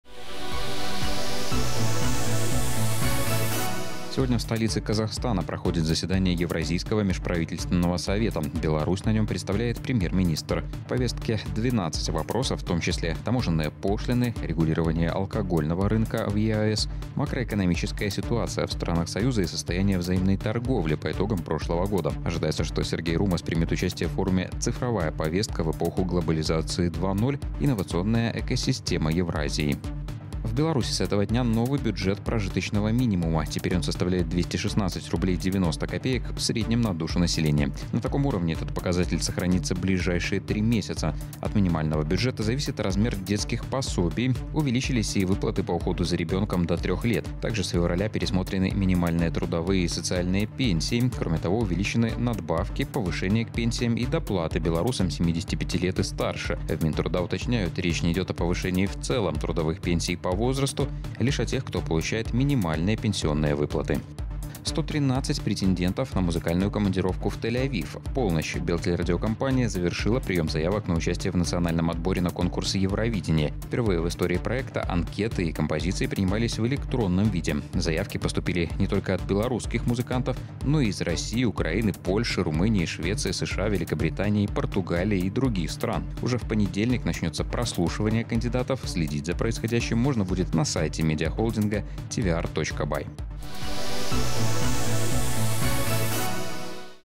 Новости Беларуси 1.02.19 10:00
Алматы принимает заседание Евразийского межправительственного совета
С февраля в Беларуси увеличиваются размеры различных видов пособий и социальных выплат в связи с ростом БПМ
Белтелерадиокомпания завершила приём заявок на участие в национальном отборе на конкурс «Евровидение»